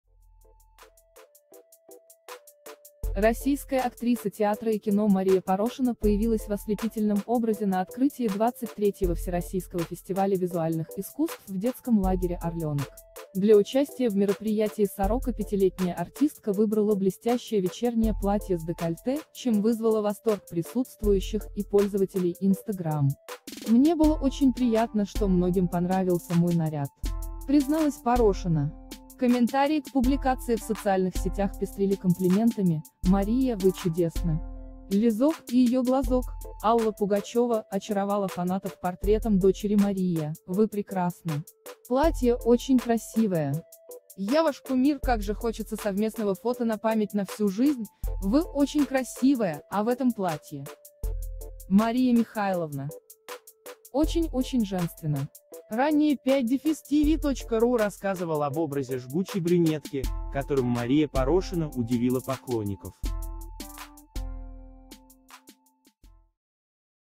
«Обалденная!»: Актриса Мария Порошина сразила поклонников вечерним туалетом
Российская актриса театра и кино Мария Порошина появилась в ослепительном образе на открытии XXIII Всероссийского фестиваля визуальных искусств в детском лагере «Орленок».

Для участия в мероприятии 45-летняя артистка выбрала блестящее вечернее платье с декольте, чем вызвала восторг присутствующих и пользователей Instagram.

«Мне было очень приятно, что многим понравился мой наряд!» — призналась Порошина.

«Мария — Вы чудесны!»;

«Лизок и ее глазок»: Алла Пугачева очаровала фанатов портретом дочери

«Мария, Вы прекрасны!!! Платье очень красивое!»;

«Я ваш кумир… как же хочется совместного фото на память на всю жизнь»;

«Вы очень красивая, а в этом платье 👍👍👍👍👍🌹🌹🌹»;

«Мария Михайловна, 😘❤️❤️❤️, очень, очень женственно)))».

Ранее 5-tv.ru рассказывал об образе жгучей брюнетки, которым Мария Порошина удивила поклонников.